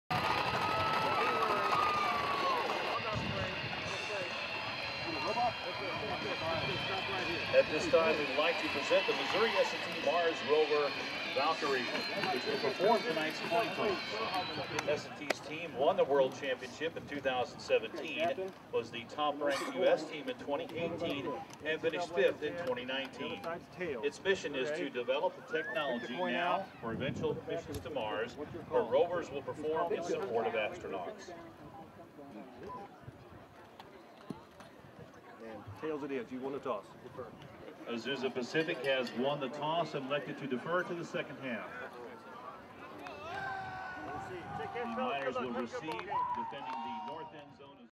Robot starts Missouri S&T football game.
The Mars Rover Design Team's 2019 rover, Valkyrie, makes the ceremonial coin flip to start the first home game of the Miners' 2019 season. For more information about Missouri University of Science and Technology, visit: mst.edu